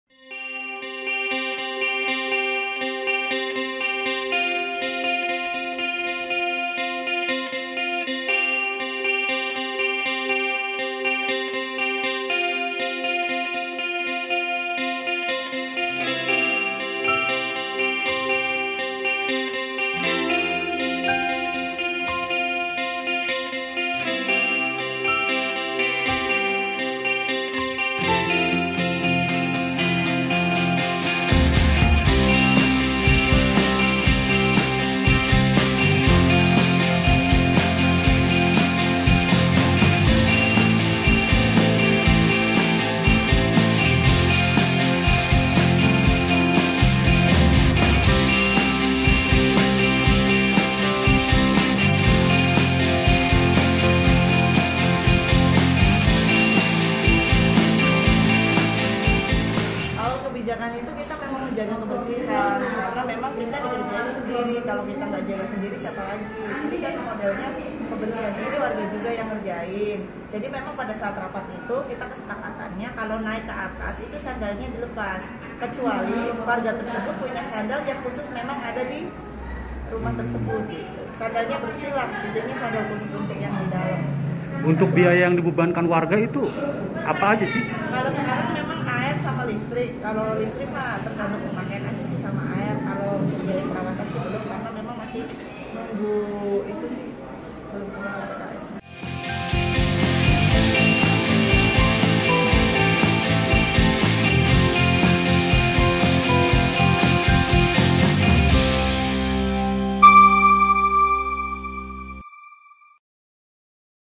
Penghuni dan Tamu Kampung Susun Akuarium Wajib Lepas Alas Kaki
Setelah diresmikan, Kampung Susun Akuarium Penjaringan langsung ditempati ratusan kepala keluarga.

Android: inews.id/android
iPhone: inews.id/ios